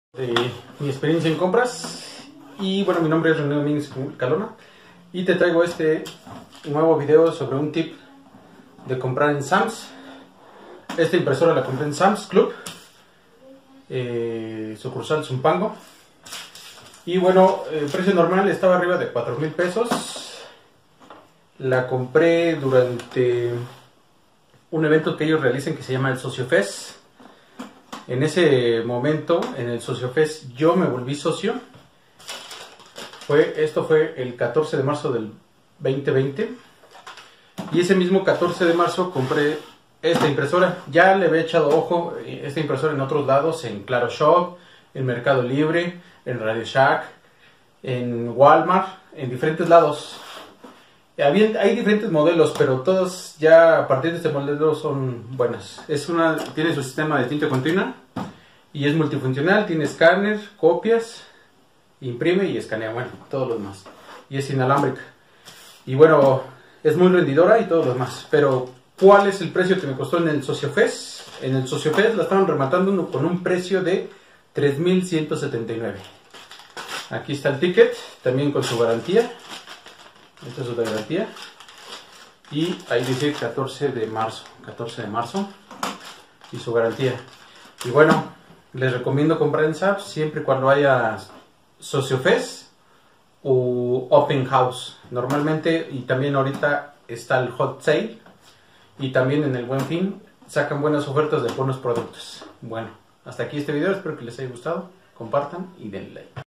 ✅ COMPRA impresora Multifuncional EPSON L4150 con TINTA Continua 604 en Sams Club
Compra de impresora EPSON modelo L4150, a un buen precio de OFERTA, en el Socio Fest de SAM´s Club

DETALLES DEL PRODUCTO EN LA PAGINA OFICIAL DE EPSON